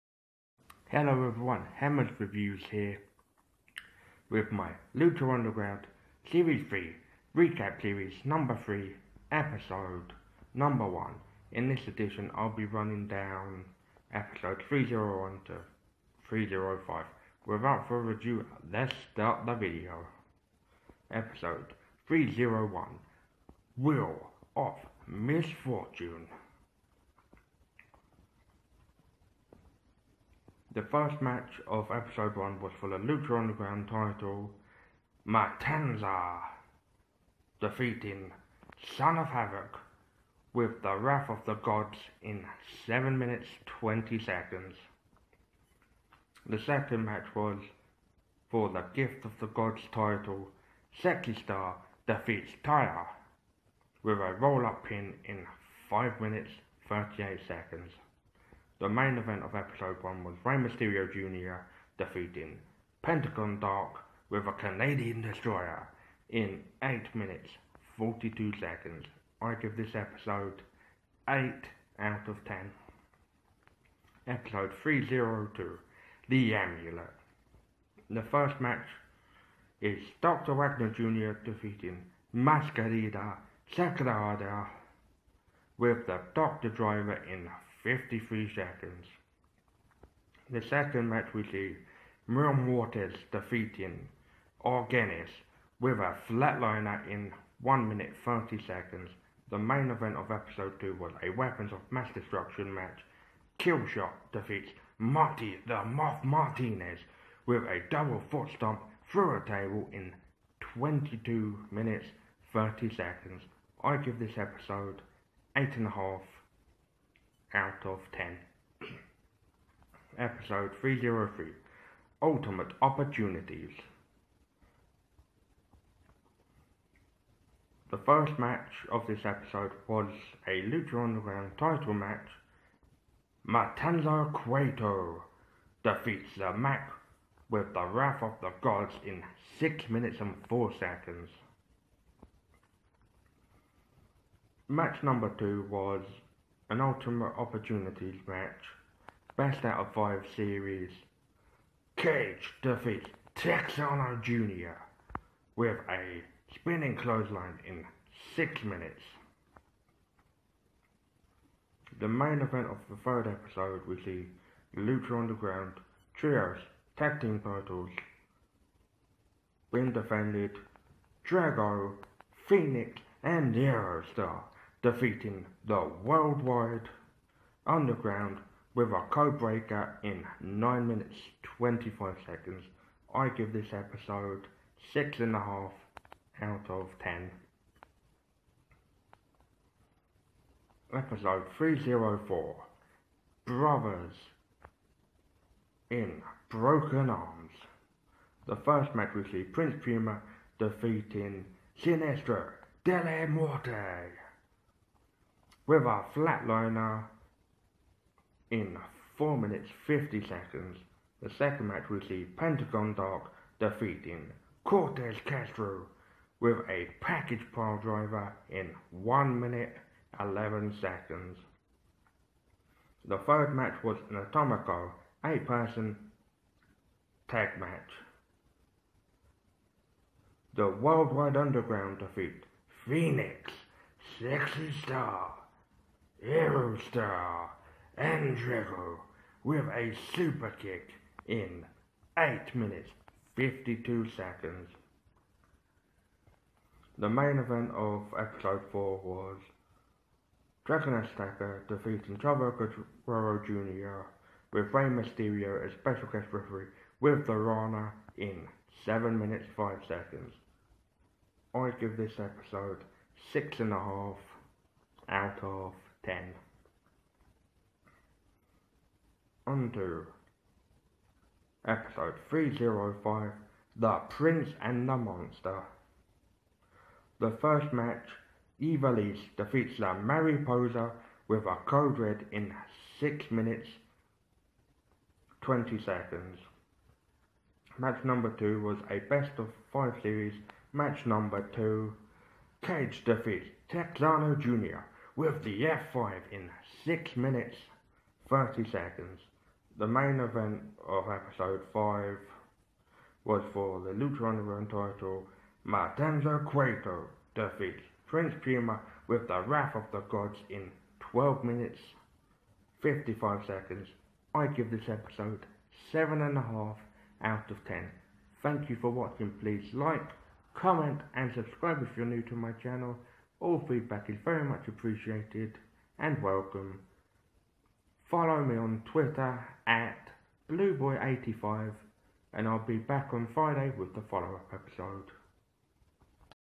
Lucha Underground Season 3 Recap Series 3 Episode 1
My lucha underground season 3 Recap Series 3 episode 1, I'm running down the results for 3.01 to 3.05

all feedback welcome